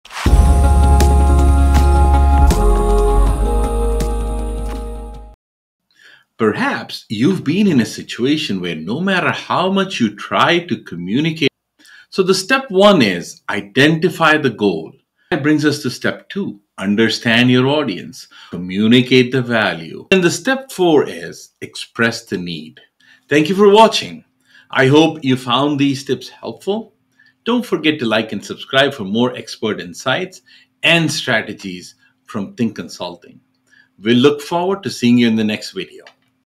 Strategic Communication t
In this video, we'll explore the concept of strategic communication and how it can help you achieve your goals. Whether you're trying to win over a love interest, land a partnership with a company, or convey your point of view to others, strategic communication is an essential tool for success.